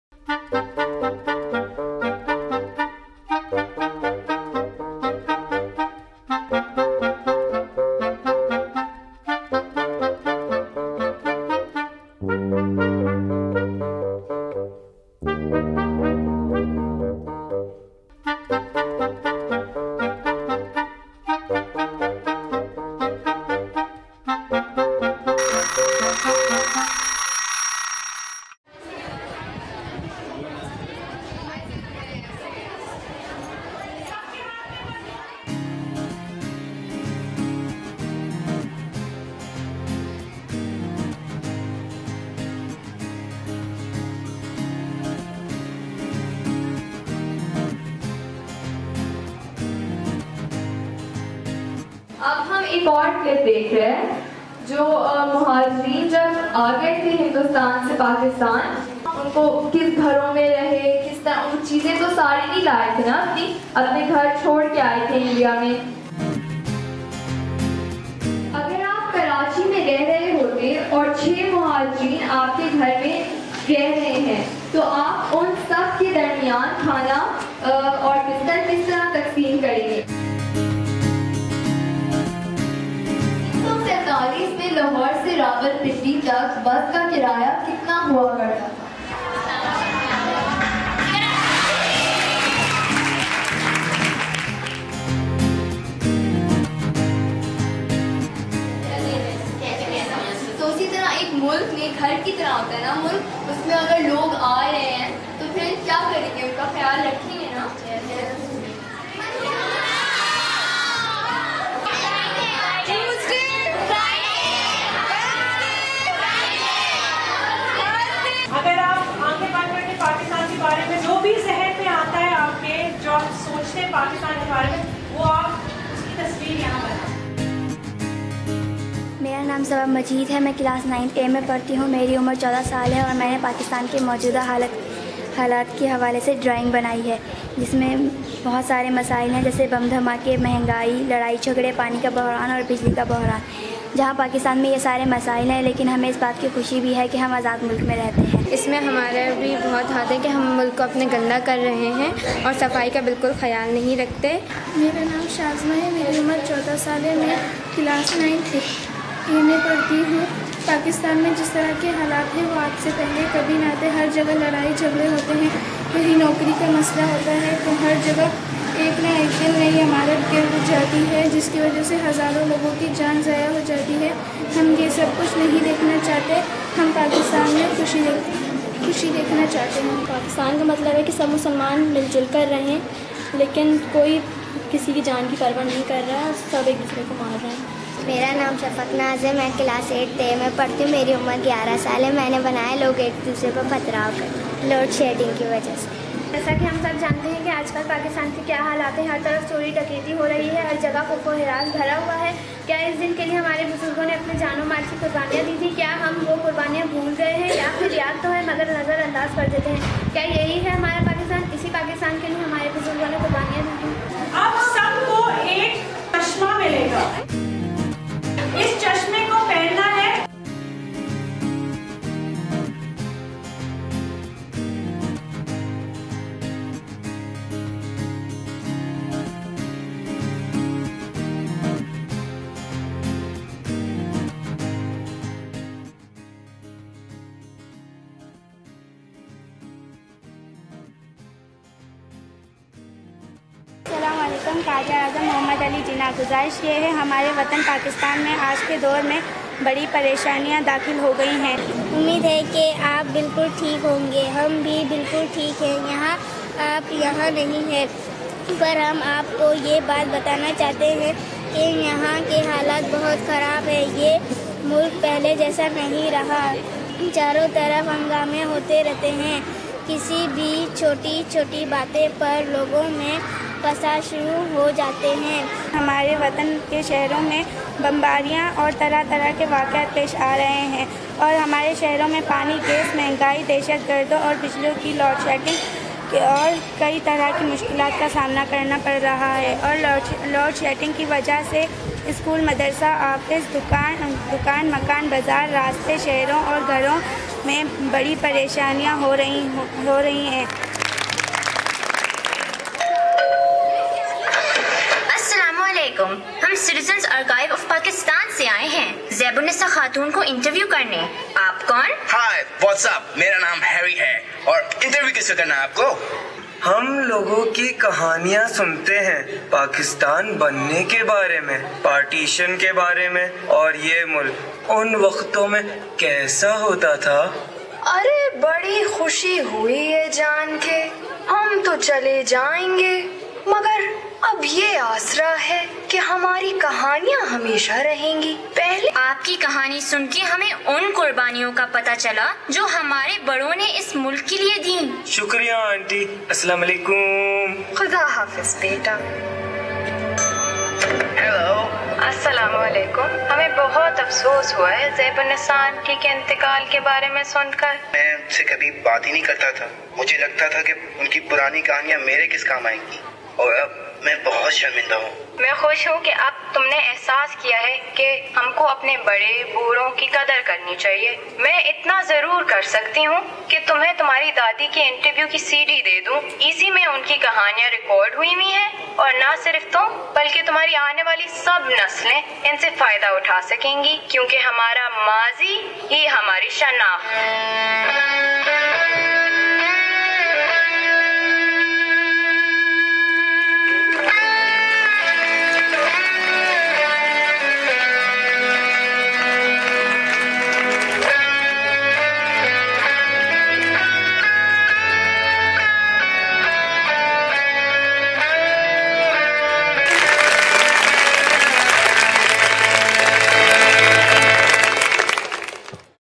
SOT - Government Girls Secondary School P and T Colony
The Citizens Archive of Pakistan brought its imagination stations to Government Girls Secondary School P and T colony where over two hundred and fifty girls heard stories of partition and early Pakistan.

Editor  - Junaid Hussain -  CAP employee